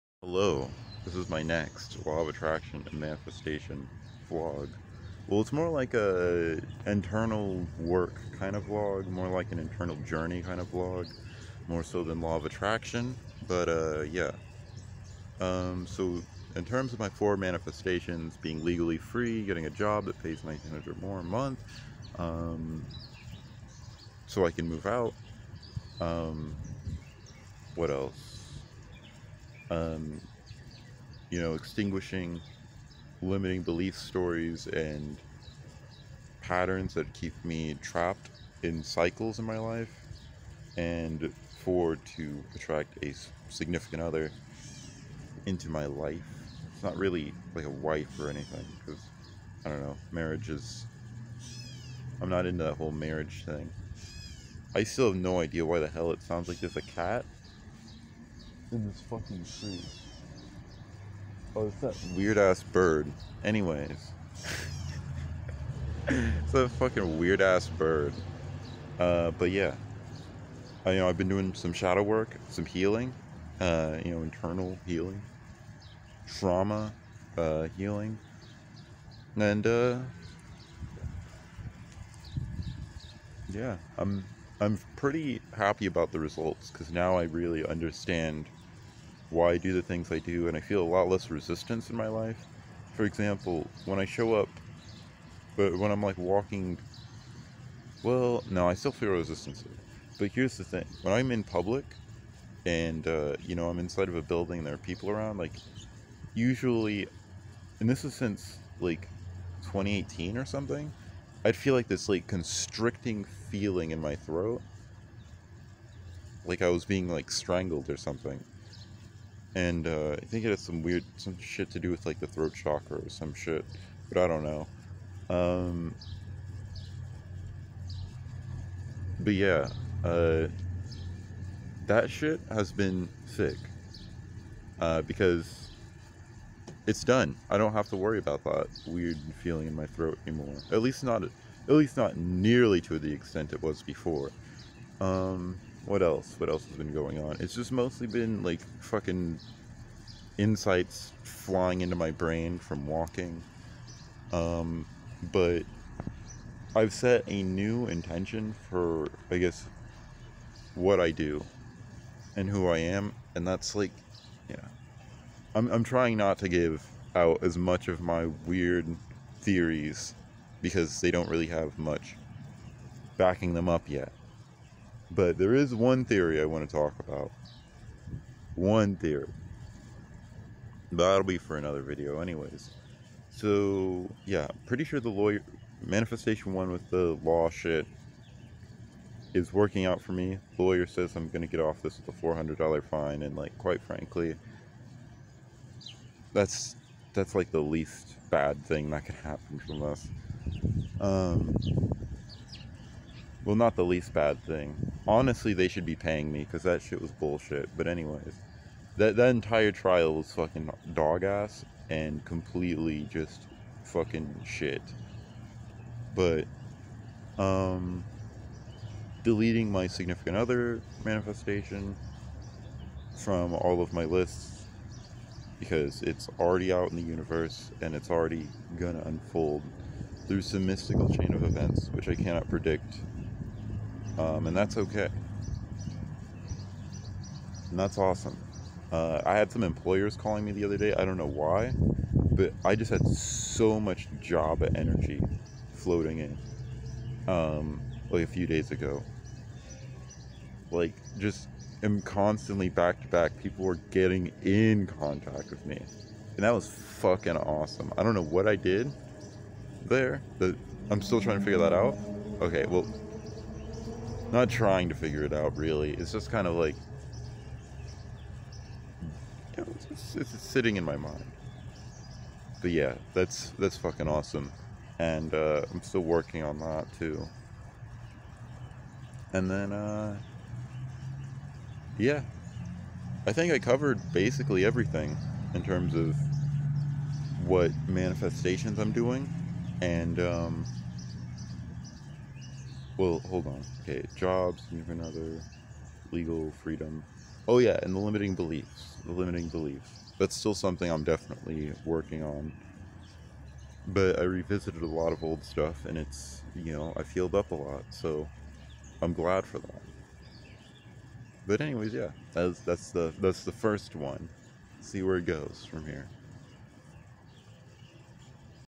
LOA Manifestation Vlog part 1
This is the first official video in my law of attraction manifestation vlog series. Will you see some crazy shit happen? Will my life make a complete fucking 180 during the course of this series? Where will your mom be next sunday evening? Who knows?